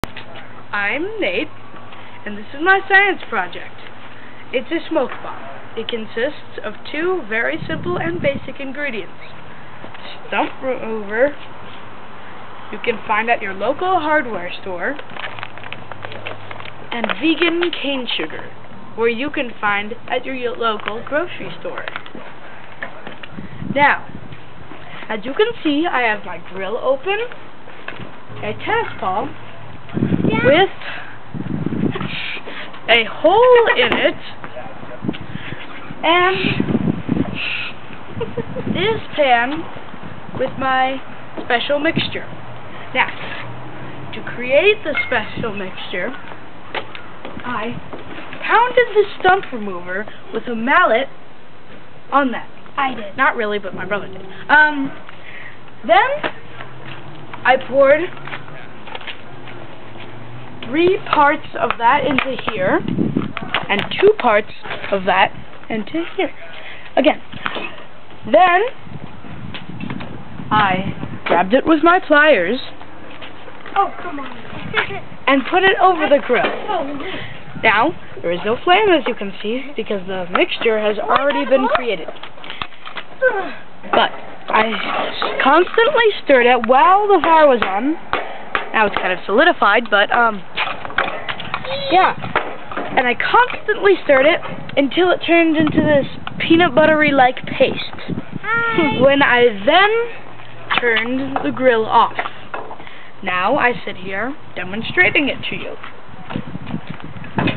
How to make a smoke bomb part 1
how to make an easy smoke bomb, good for science projects at school, also.